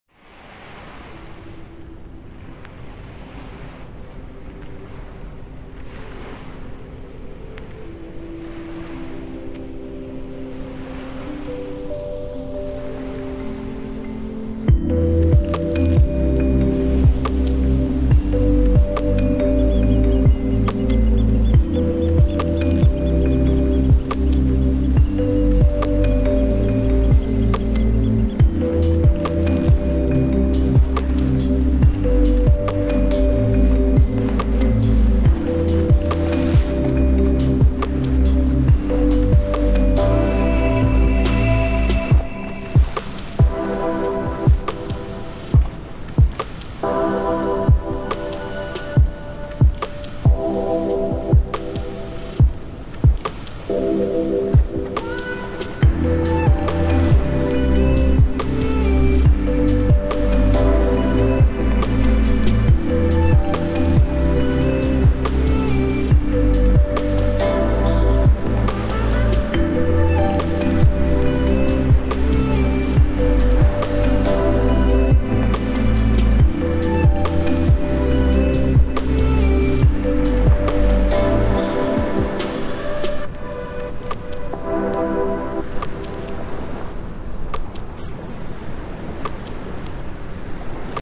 Sony A7c + Helios 44-2 Anamorphic mod
Trying to achieve an anamorphic look on a budget.